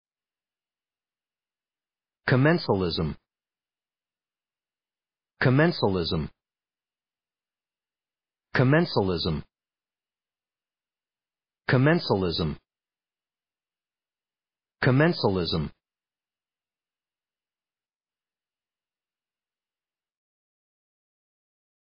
How to pronounce commensalism
commensalism American English pronunciation. How to pronounce commensalism correctly. How to say commensalism in proper American English.